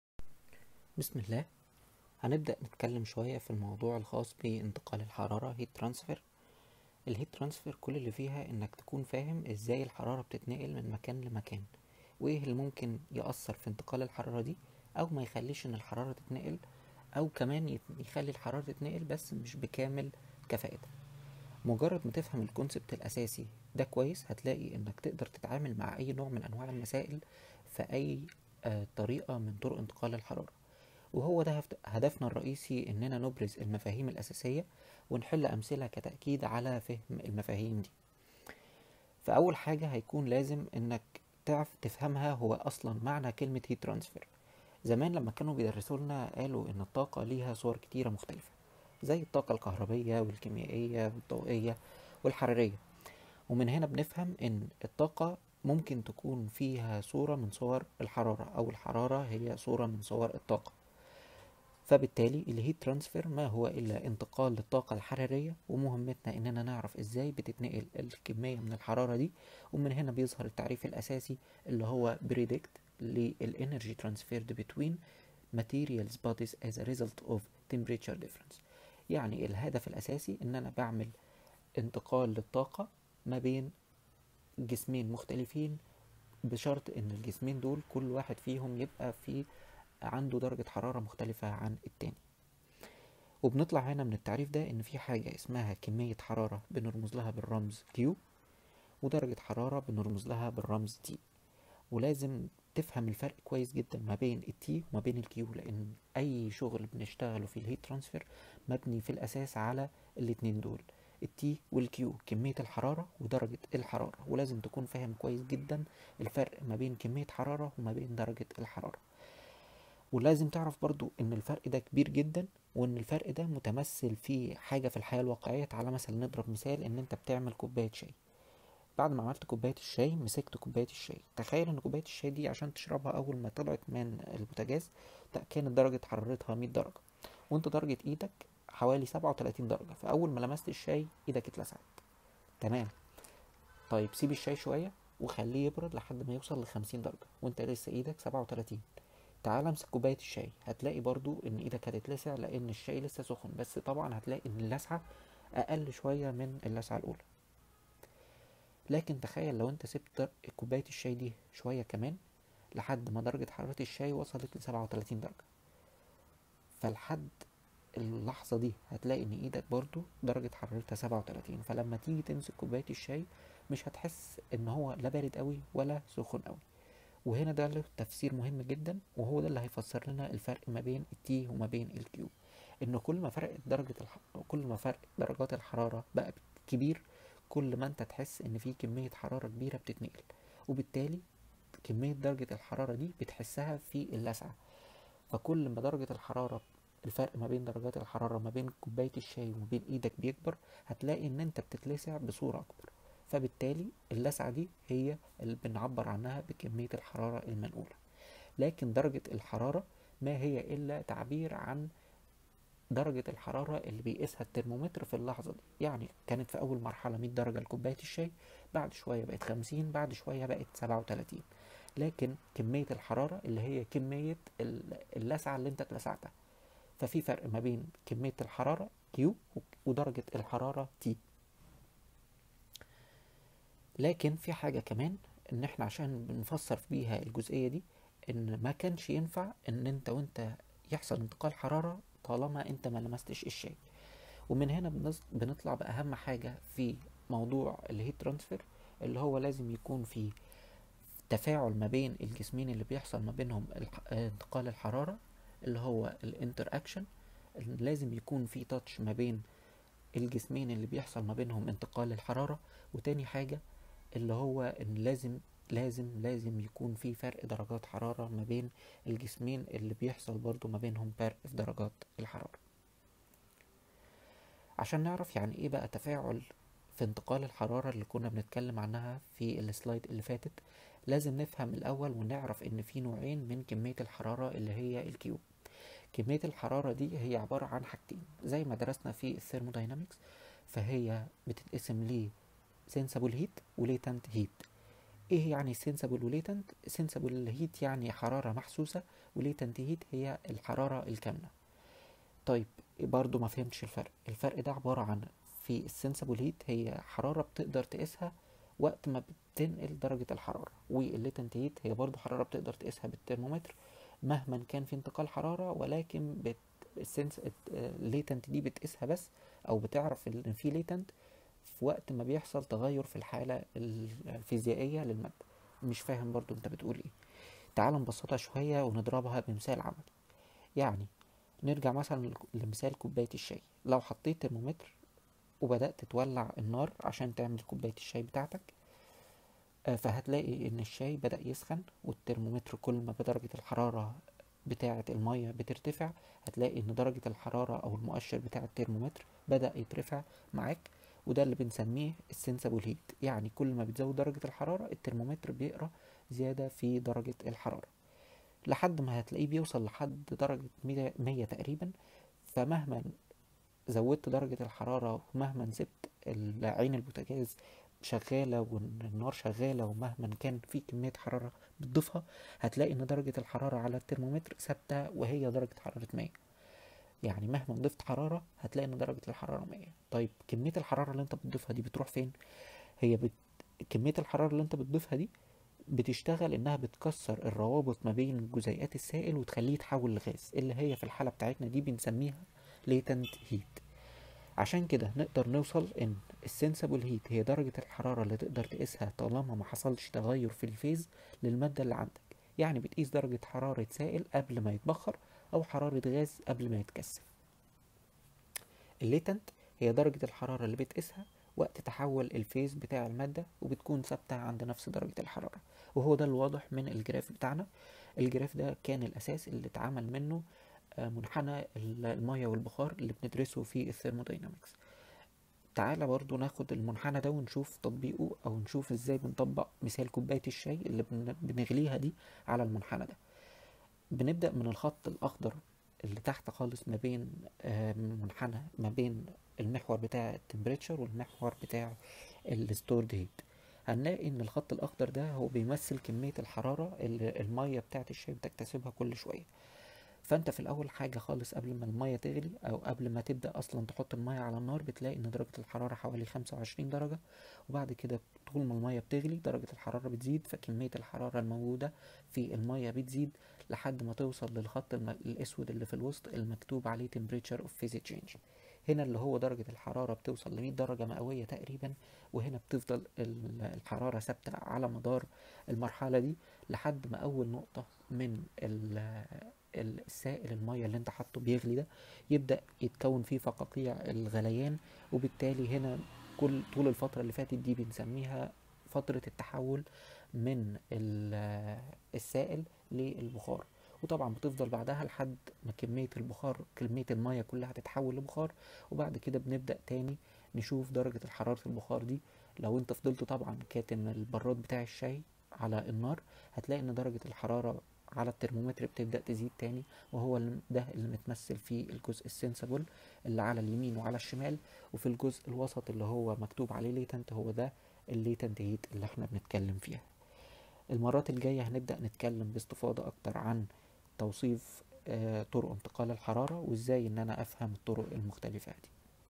١. مقدمة عن كورس انتقال الحرارة و الكتلة || Introduction to heat and mass transfer
مقدمة عن
1.الفرق بين درجة الحرارة و كمية الحرارة
2.الفرق بين الحرارة الكامنة و الحرارة المحسوسة
3.تعريف بسيط بمادة انتقال الحرارة
Introduction to :
1.Heat transfer concept
2.Difference between amount of heat and temperature
3.Difference between latent heat and sensible heat